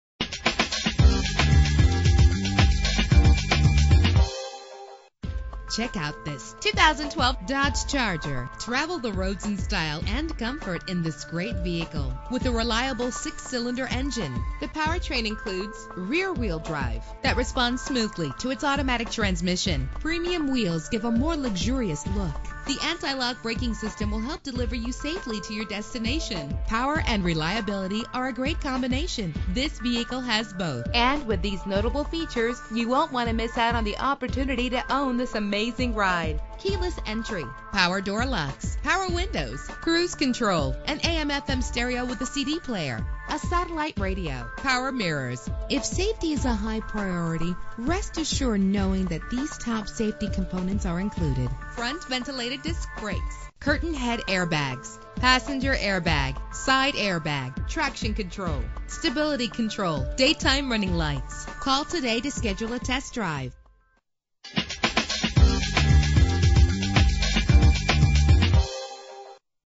2012 Dodge Charger - Ontario CA
Year: 2012
Make: Dodge
Model: Charger
Trim: SE
Engine: 3.6 liter 6 cylinder 24 valve
Transmission: Automatic
Color: Tungsten Metallic
Mileage: 26704
Address:
701 S Katering